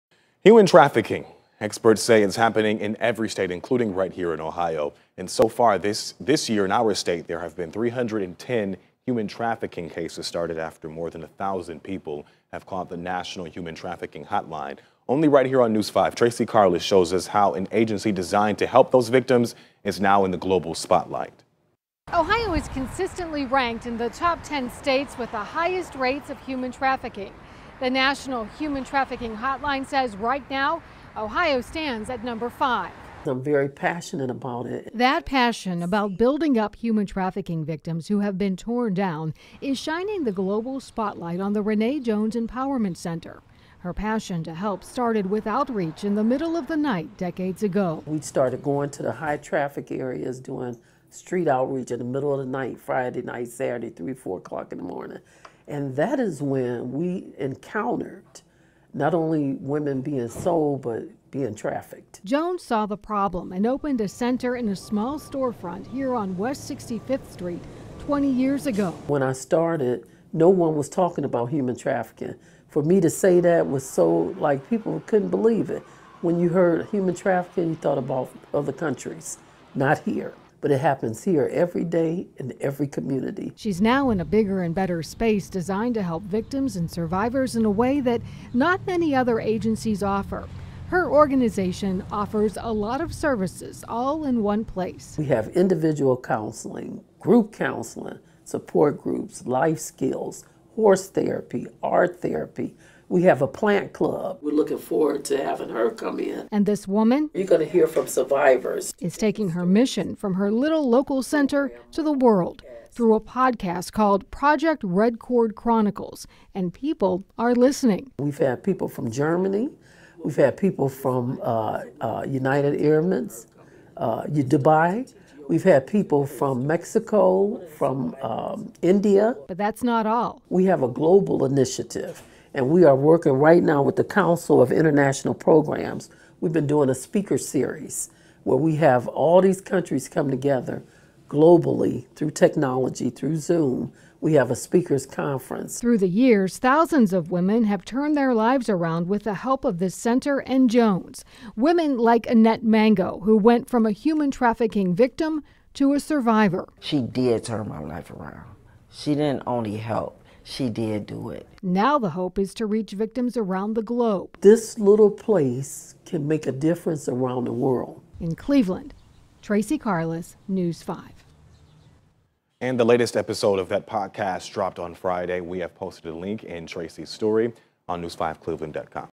Renee Jones Empowerment Center starts podcast aimed at helping victims of human trafficking
The Renee Jones Empowerment Center in Cleveland is taking its message about helping human trafficking victims to a global audience with a new podcast.